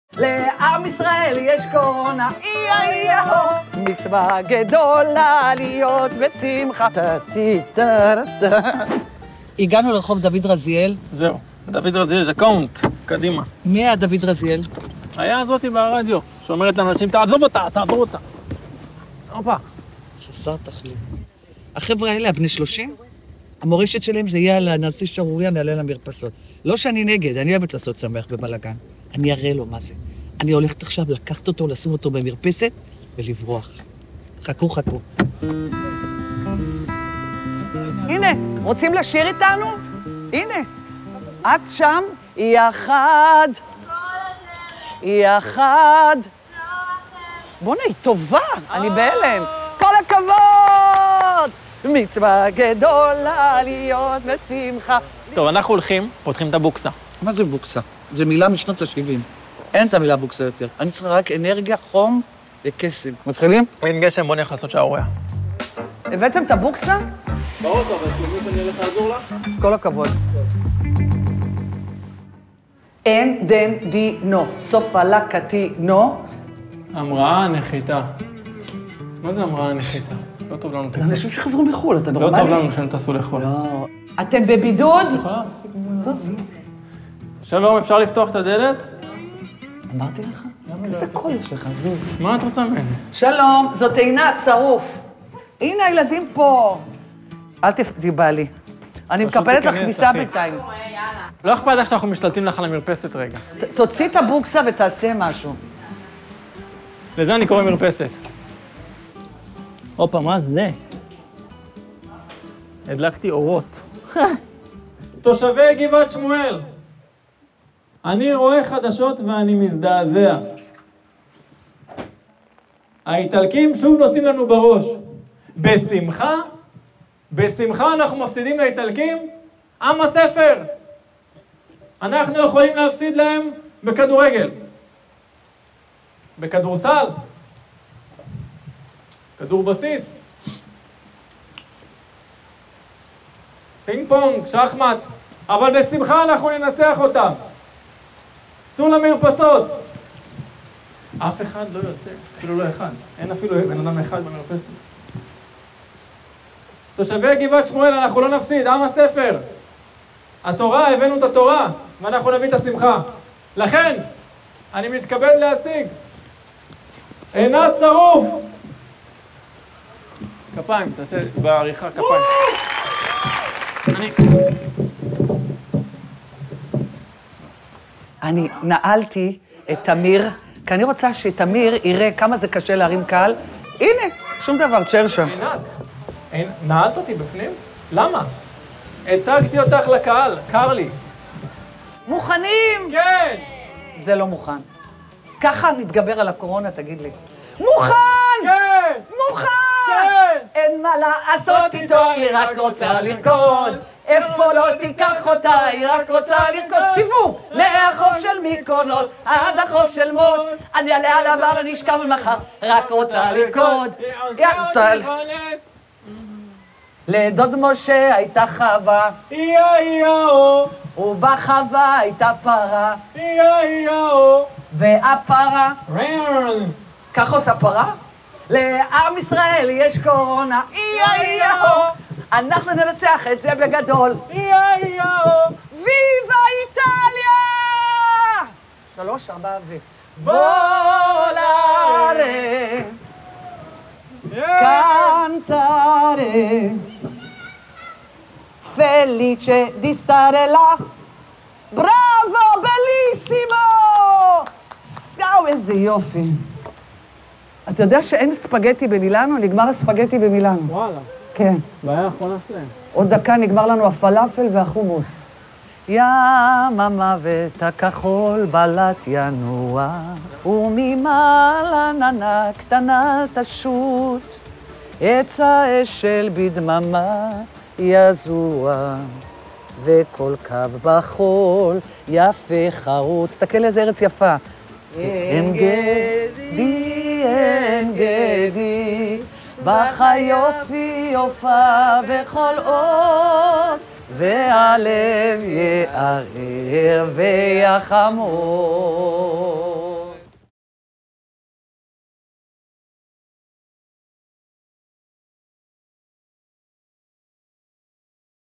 כשתמיר בר הביא את עינת שרוף לשיר במרפסות
כאן | דיגיטל - אחרי איטליה וספרד, דואגים לכם עם גרסה מקומית של "שירת המרפסות". ומי יותר מתאימה למשימה מכוהנת השירה בציבור עינת שרוף. על הצהלולים: תמיר בר

הגשה: תמיר בר
צילום: רועי קייזרמן, רן קושניר
הפקה: נעמה רוזן, טל רבינובסקי
ע. הפקה: שלומית מסאלה
עריכת וידיאו: אלכס חוסיד